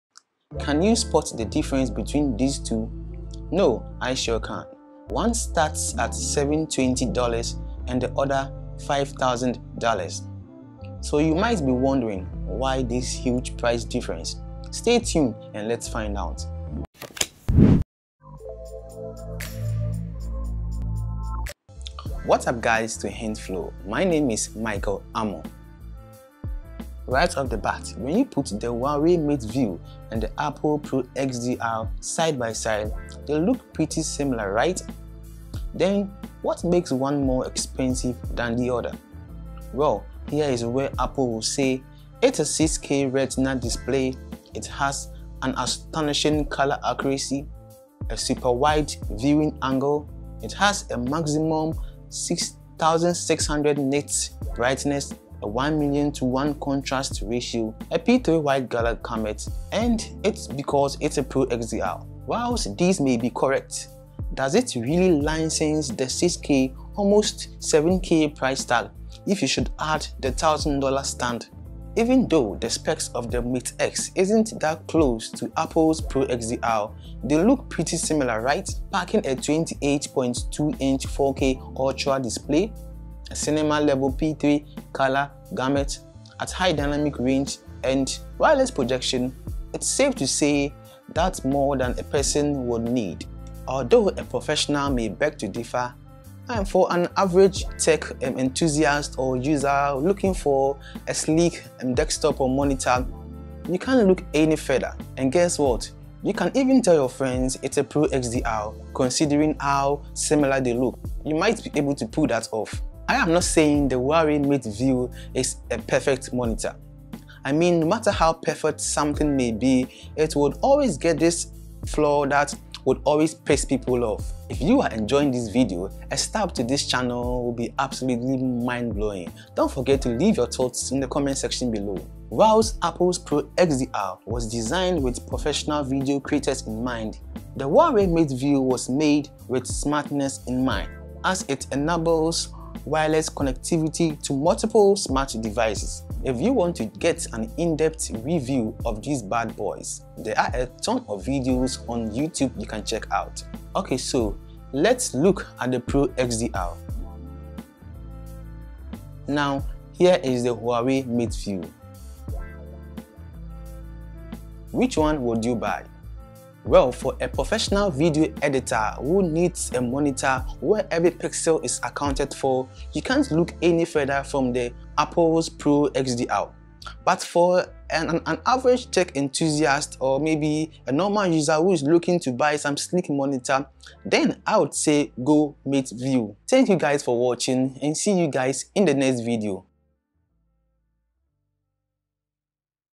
$7,000 VS $700 MONITOR. WHICH ONE WILL YOU CHOOSE?
These two monitors look similar but i bet you their price different will blow your mind. Leave your though in the comment section below. Is there any product you will like to see me do a video on.